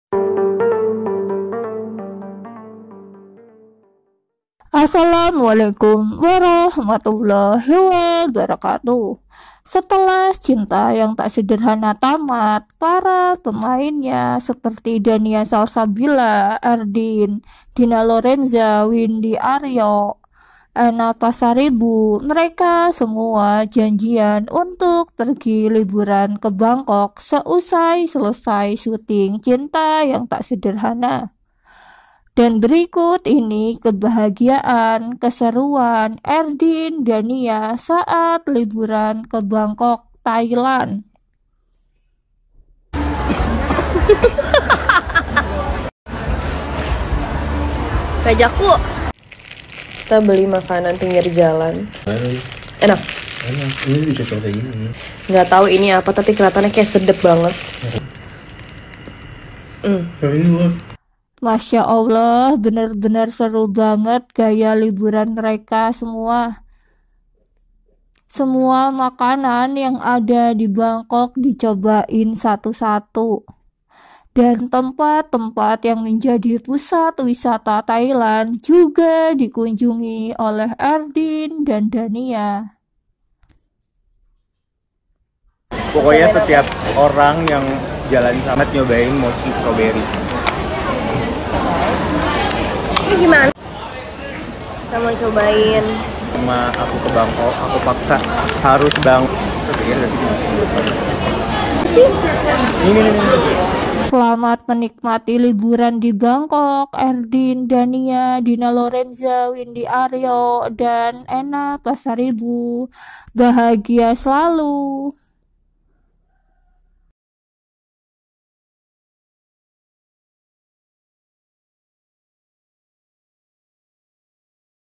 Kebahagiaan dan Keseruan Dannia Erdin Dina Lorenza Ena Windy Ario Saat Pergi Liburan Bersama
Pergi Liburan ke Bangkok Setelah Cinta Yang Tak Sederhana tamat,, semoga Dania Salsabilla Erdin Windy ario ena Dina lorenza Bahagia waktu Liburan